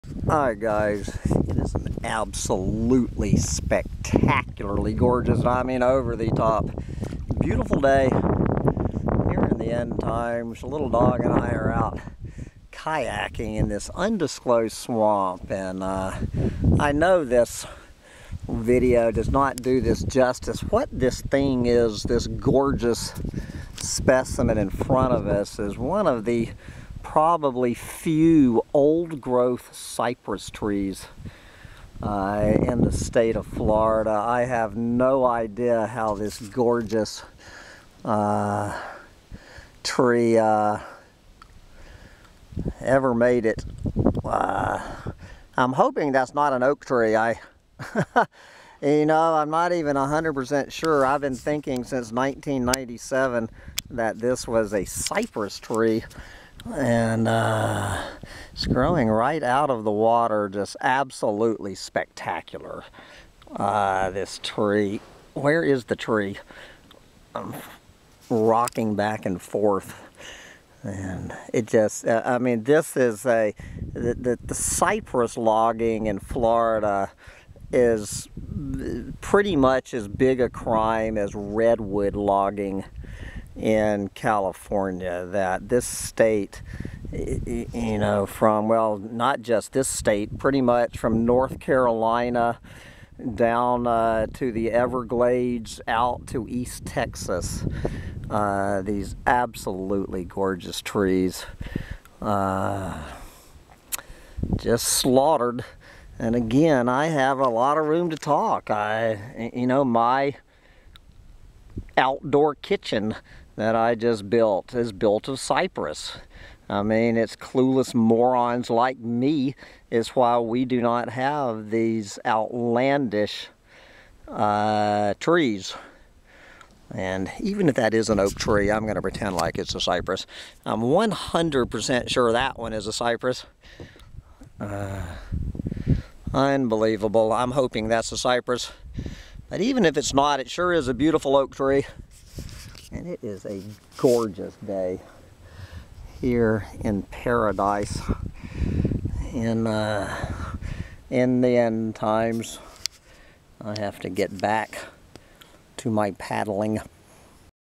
A Peek Into What Florida Should Still Look Like
I did confirm that was, indeed, an old-growth cypress tree. What a wasteland we have made of this Garden of Eden.